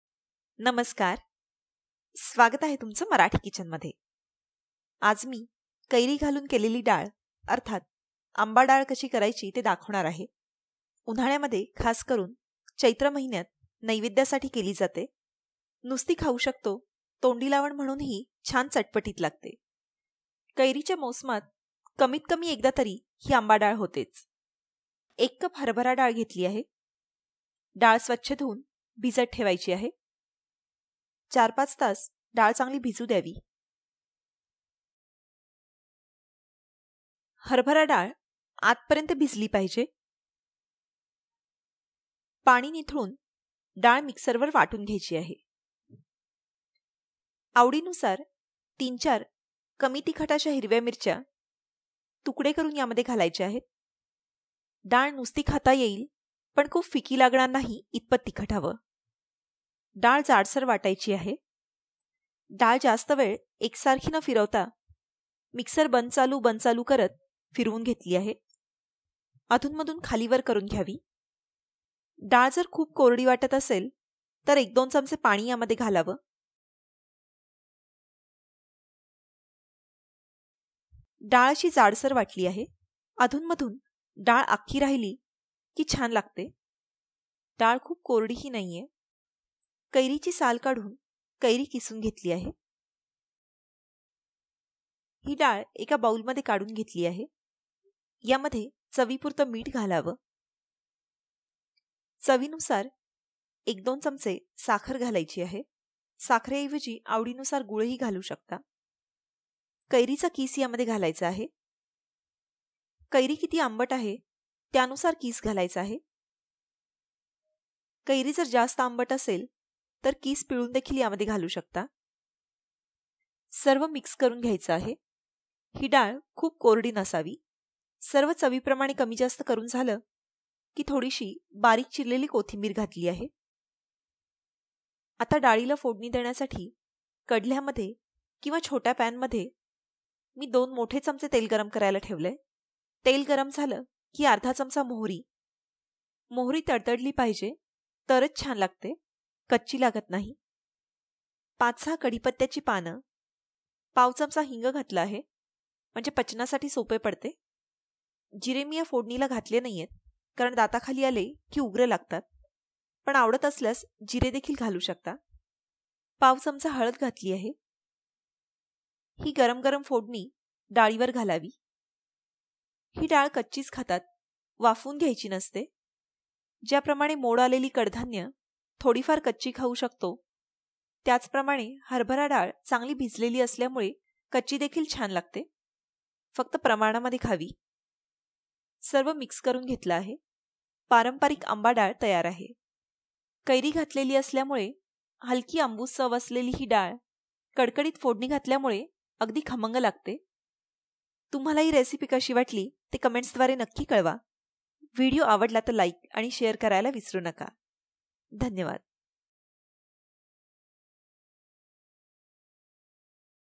पारंपारिक आंबाडाळ अर्थात कैरीची डाळ | Kairichi Dal | Amba Dal
साहित्य
१ कप हरभरा डाळ
१ कैरी
१-२ चमचे साखर
मीठ
कोथिंबीर
3-४ हिरव्या मिरच्या

फोडणीसाठी
मोहरी
हिंग
कढीपत्ता
हळद

Ingredients:
1 cup Chana Dal
1 Raw Mango
3-4 Green Chilies
Salt
1-2 tsp Sugar
Coriander Leaves

For tempering
Mustard Seeeds
Curry Leaves
Asafoetida
Turmeric